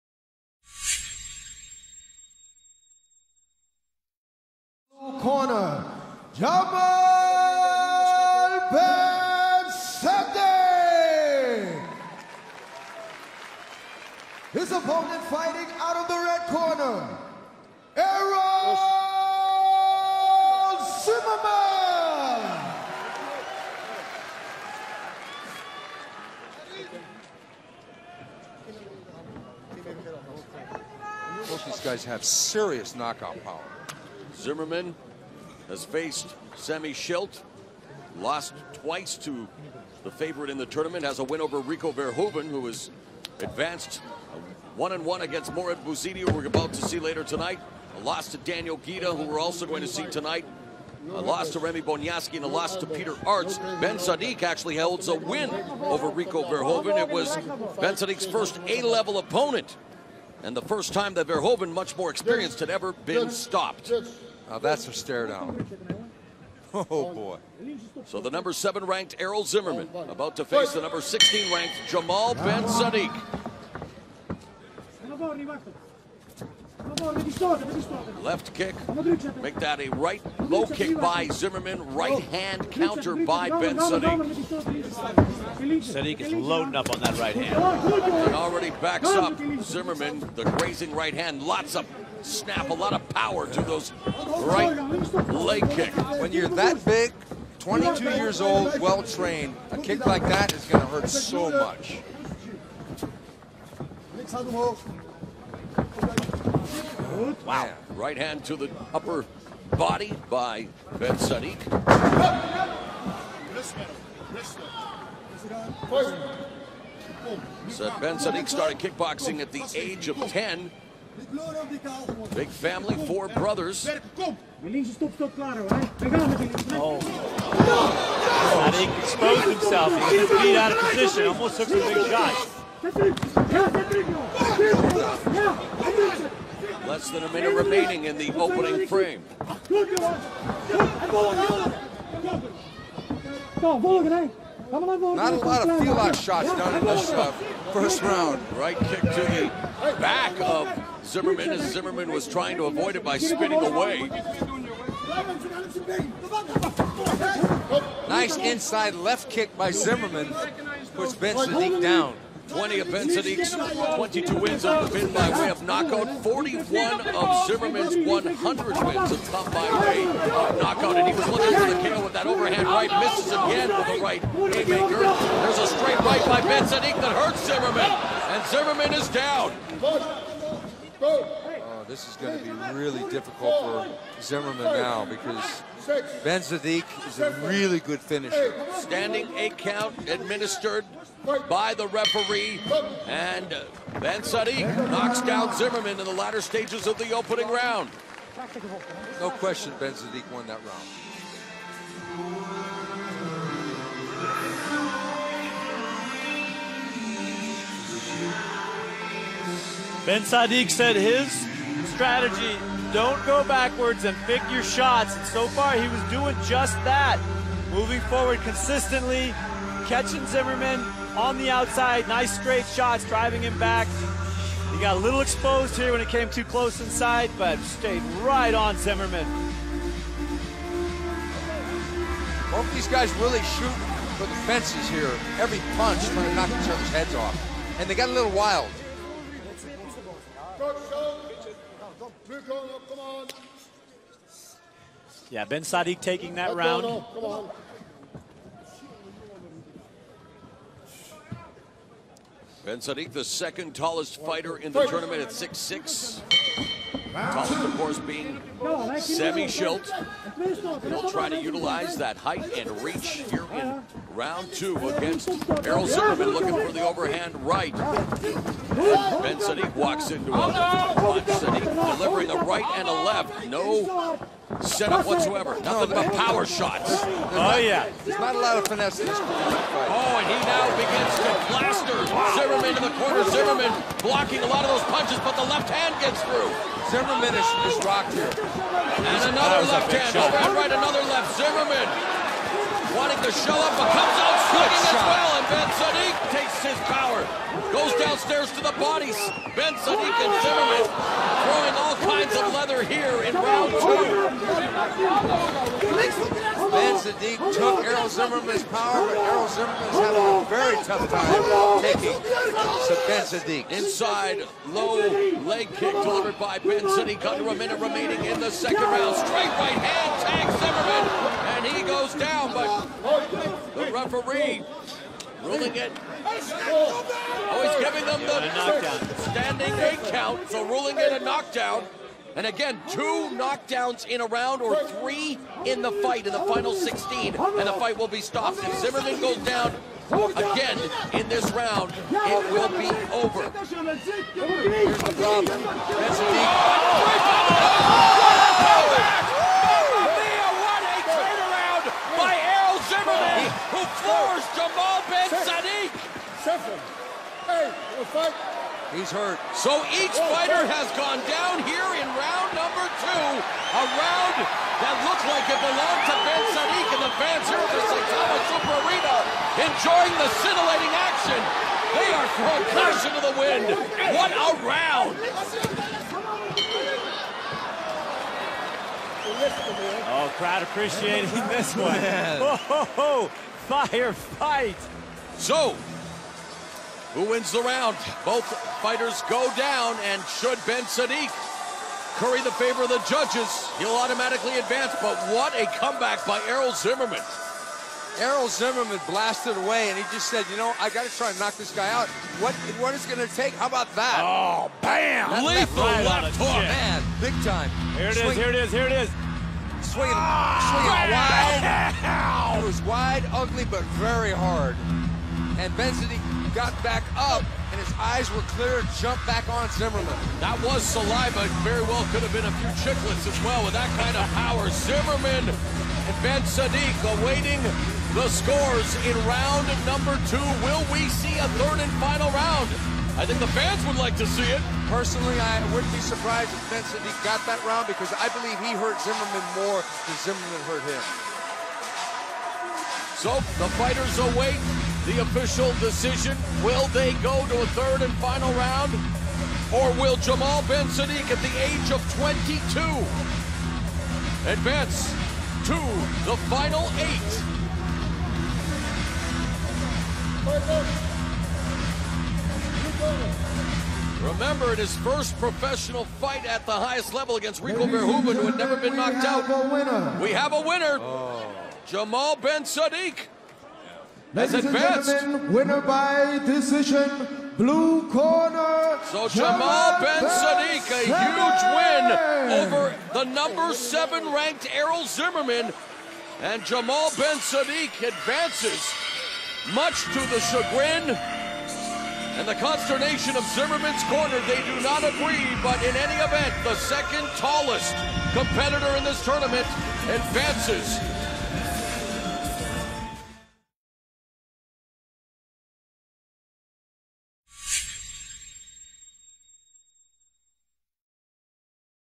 GLORY 4 Tokyo - Errol Zimmerman vs. Jamal Ben Saddik (Full Video)
Full video of Errol Zimmerman vs. Jamal Ben Saddik at GLORY 4 Tokyo in December 2012